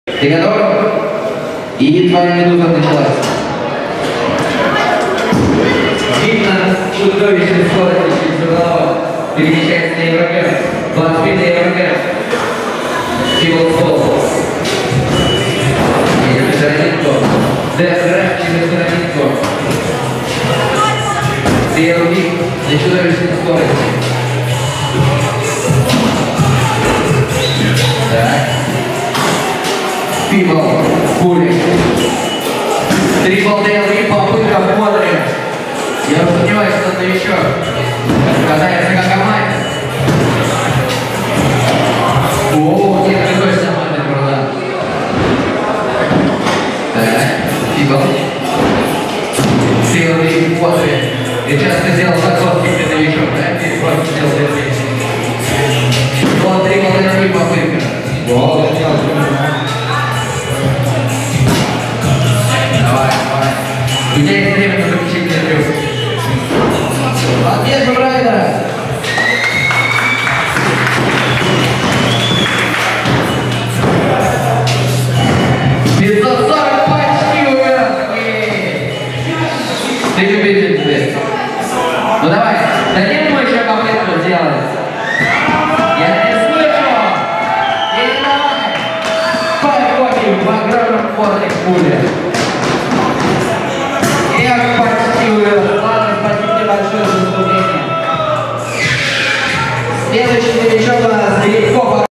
Прокофьев Павел - kick scooter контест САБОТАЖ в скейтпарке Sababa 20161119
19 ноября 2016 в скейтпарке Sababa при участии магазинов ScooterUnity и DOSKI SHOP прошел контест для новичков и любителей SABOTAGE.